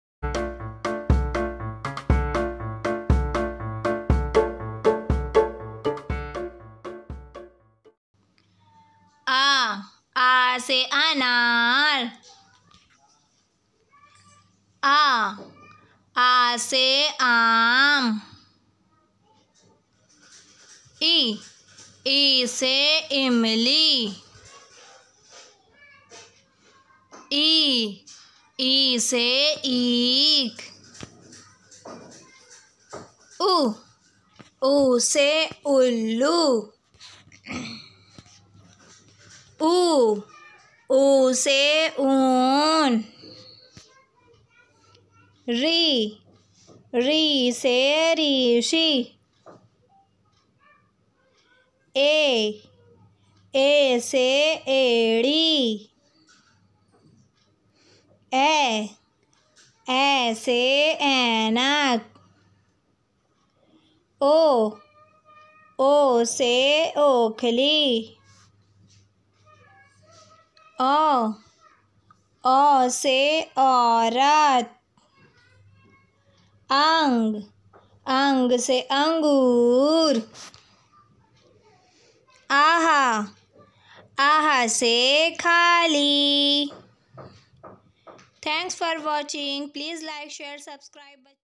a se anar, aa se aam, e se emli,अ से अनार, आ से आम, इ से इमली
अ से अनार, आ से आम, इ से इमली, ई से ईख,
छोटे बच्चों के लिए अ से अनार से अः  तक वीडियो तैयार की गई है बच्चों आप लोग चाहिए उसे learn कीजिए read करिए और अगर आप लोगों को मेरी वीडियो अच्छी लगे तो प्लीज मेरे वीडियो को लाइक शेयर सब्सक्राइब करना बिल्कुल नहीं भूले ओके बाय-बाय 🤗😘